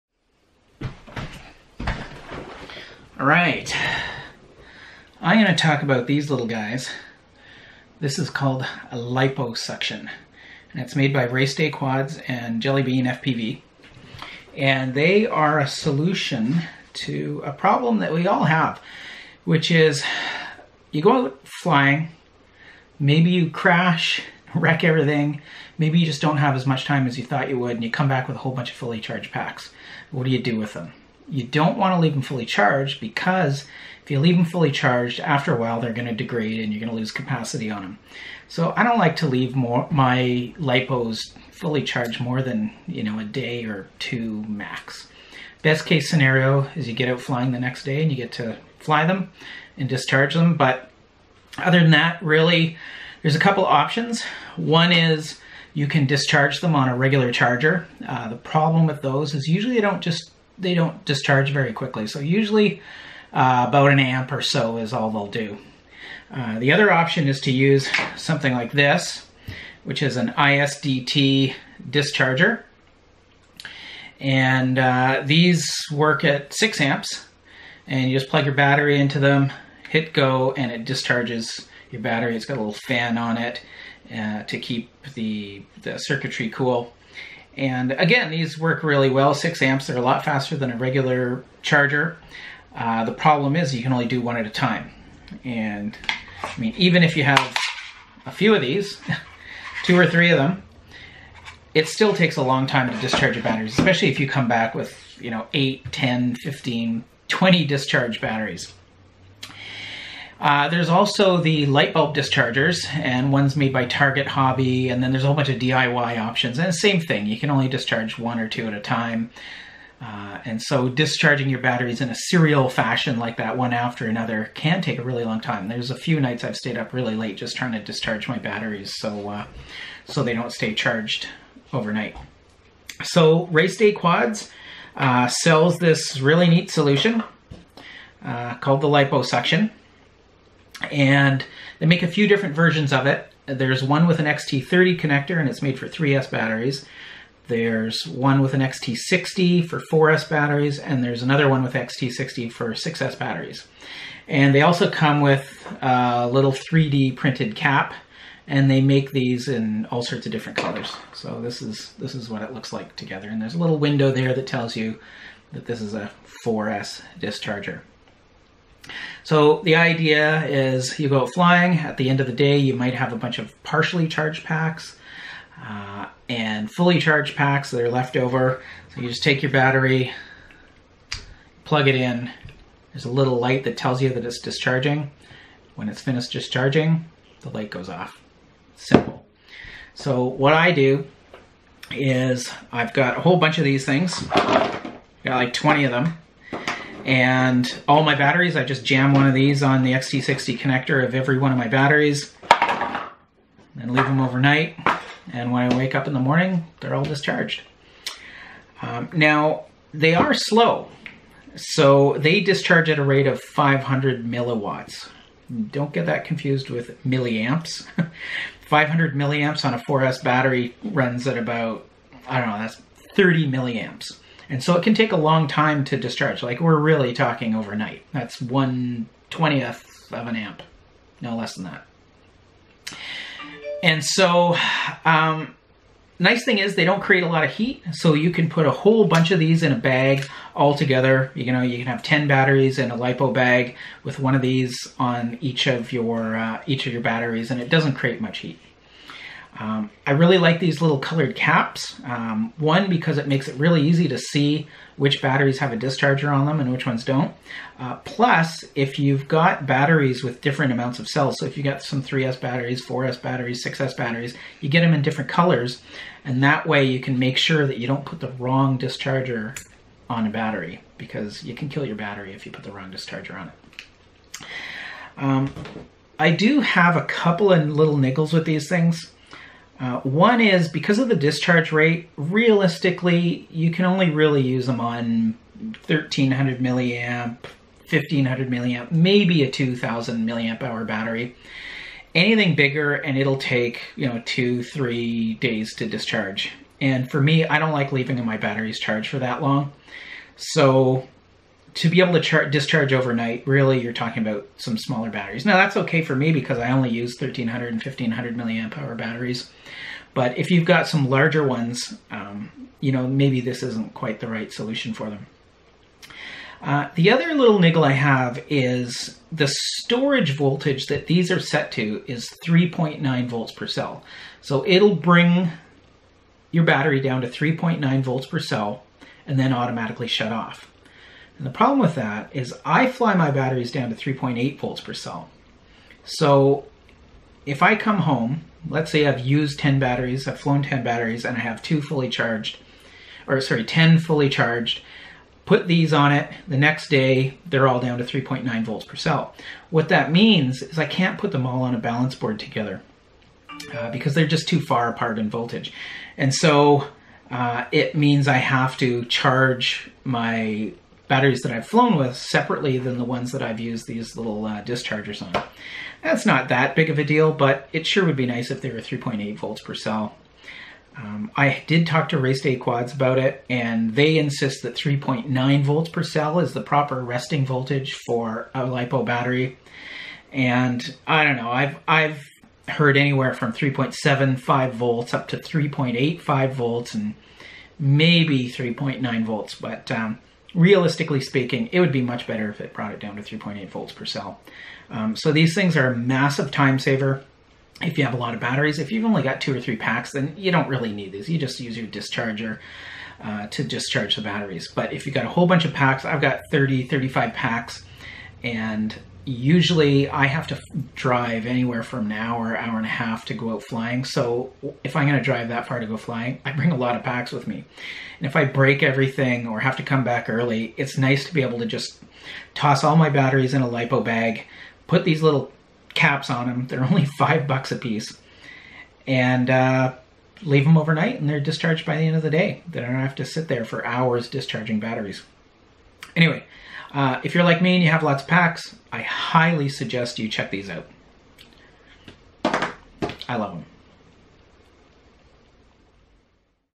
RDQ Lipo Suction battery discharger REVIEW!
Review of the Lipo Suction battery discharger from RaceDayQuads.  These are used to discharge unused lipo packs back to storage voltage to keep your unused batteries in good shape.